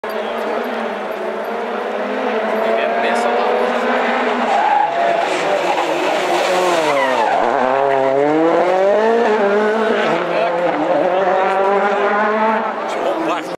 Ford Fiesta RS WRC Nice Hairpin Turn [HD]
Bernhard Ten Brinke on the limit in the ELE rally 2012.
Nice hairpin slide.